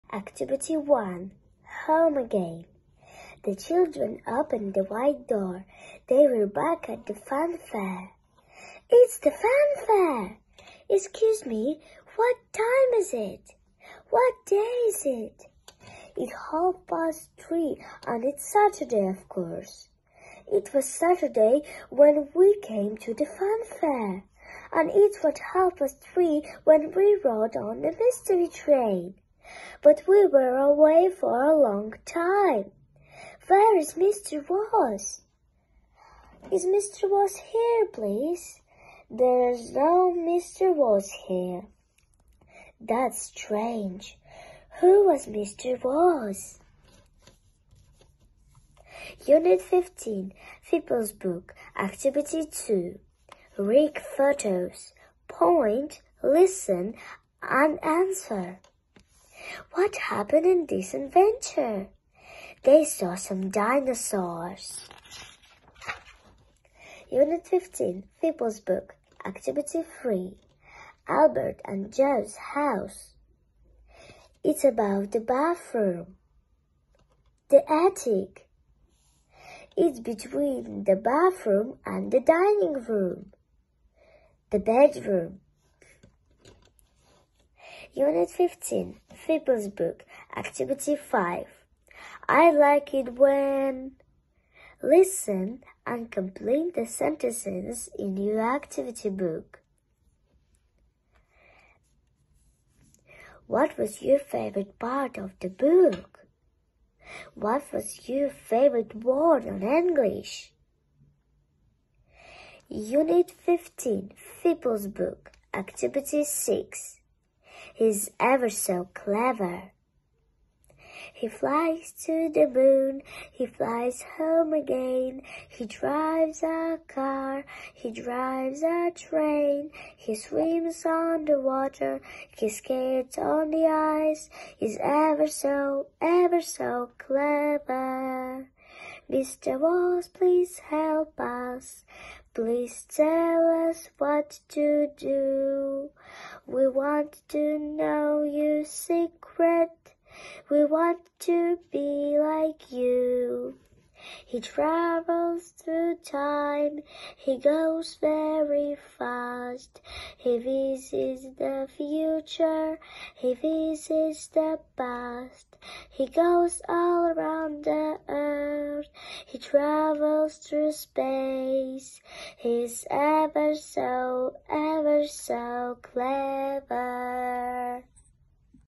Mary - reading (Excellent unit 15)
Mary has been studying English for 1.5 years.